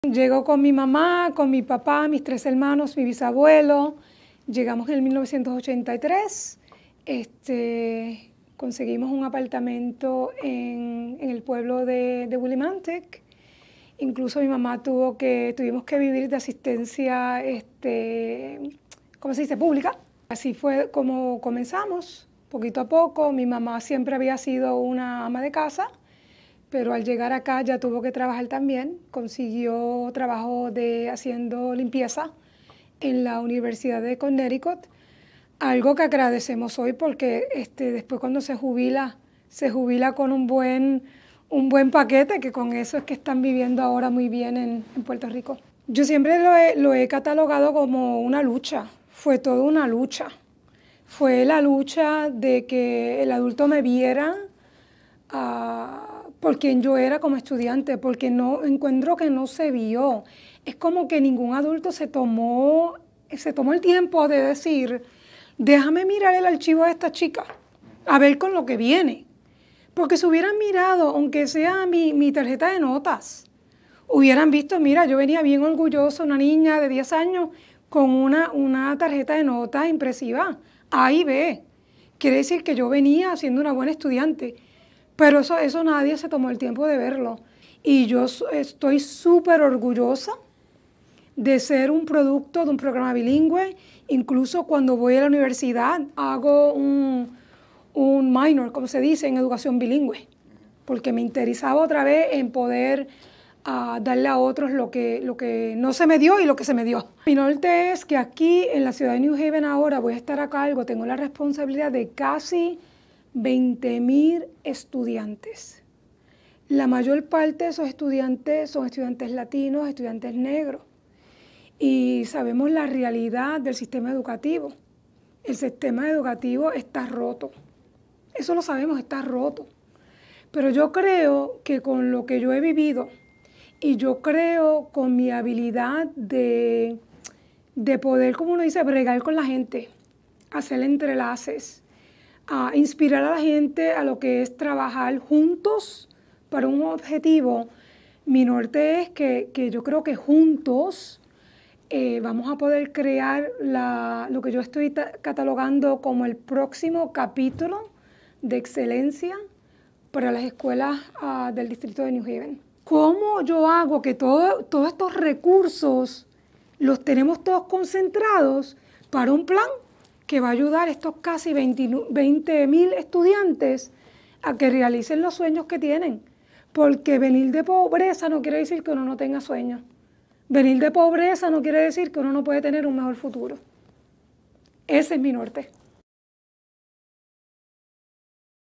Conoce a Madeline Ramos: boricua es nueva jefa de escuelas públicas de New Haven
Cuenta cómo se convirtió en líder de casi 20,000 estudiantes en esta ciudad de Connecticut.